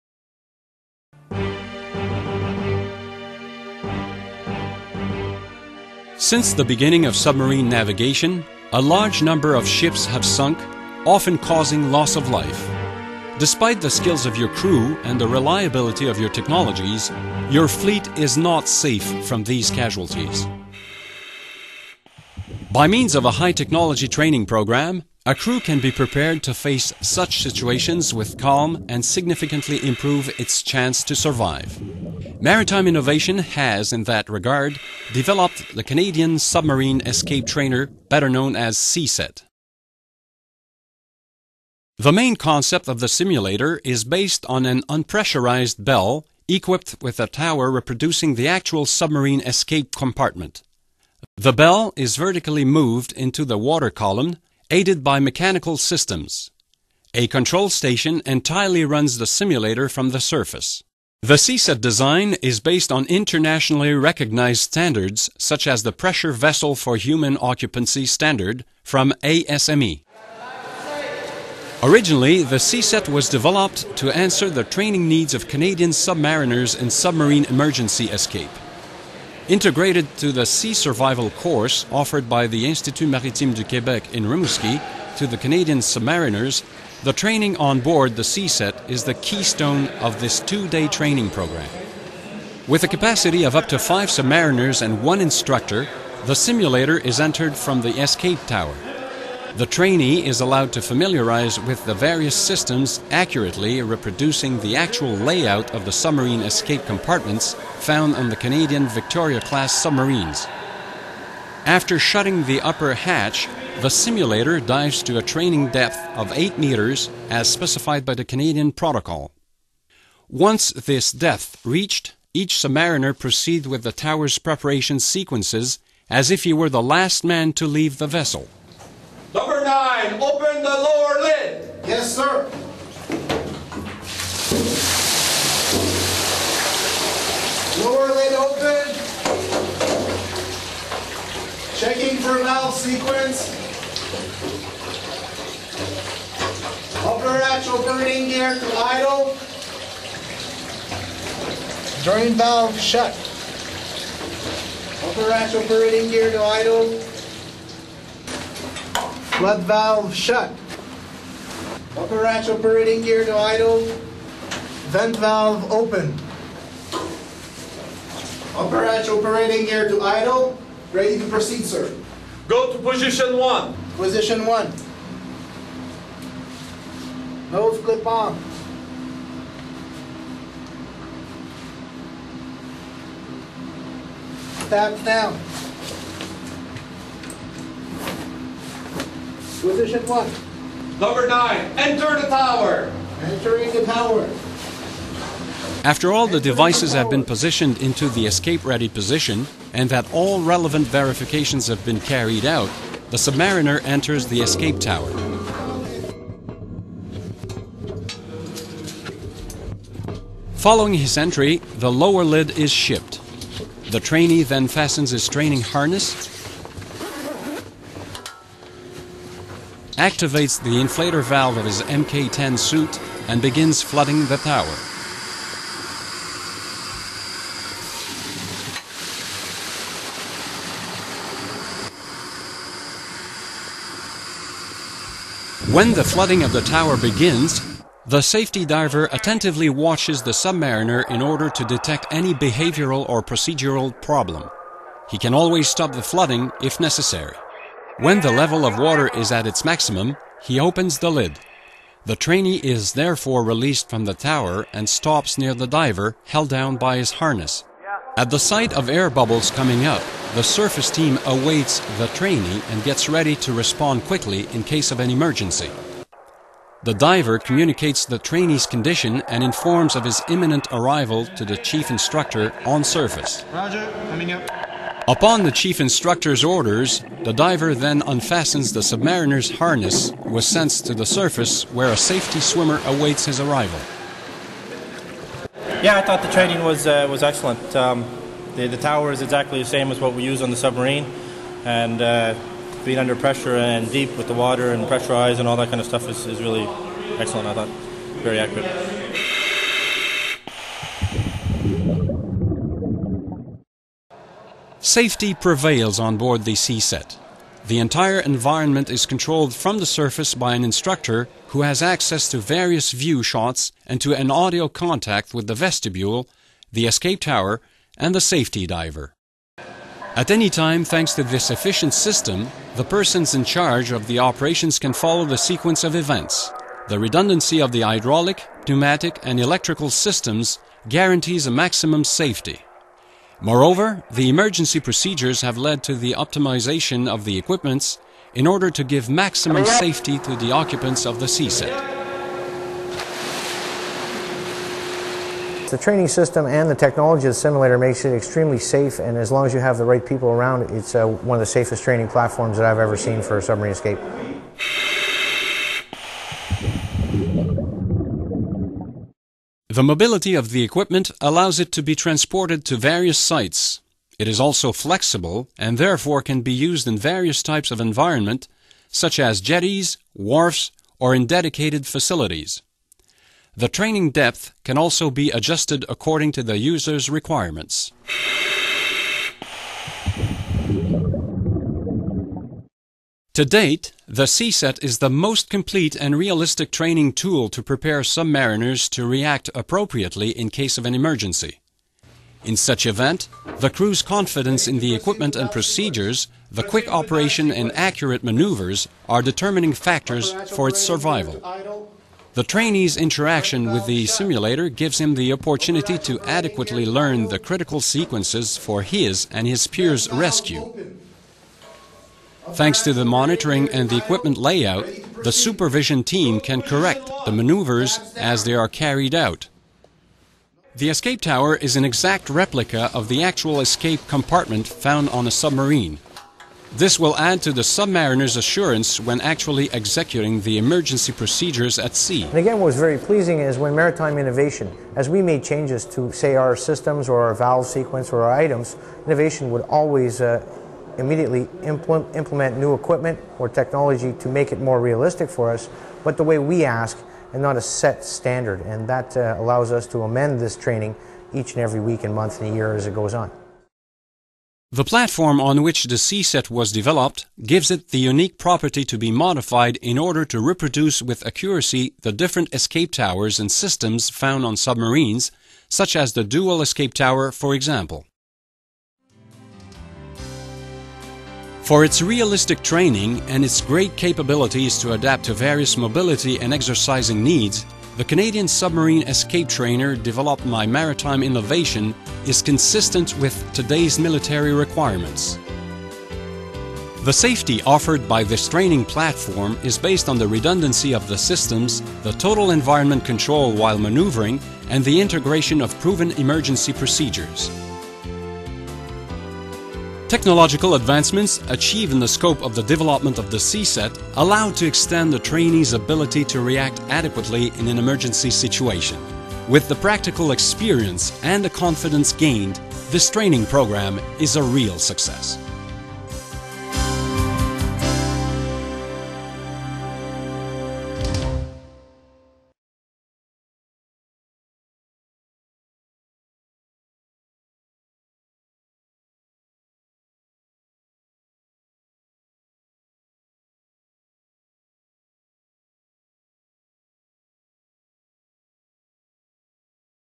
The Submarine Escape Trainer
Innovation maritime in collaboration with the Institut maritime du Québec (IMQ) and the Canadian Forces Naval Operations School, developed a unique emergency escape simulator, named the Submarine Escape Trainer (SET). It provides a realistic, credible and safe platform for this type of practical training.

The SET is a diving bell equipped with a replica of a submarine escape tower. This bell is moved vertically in a water column with the help of a mechanical system.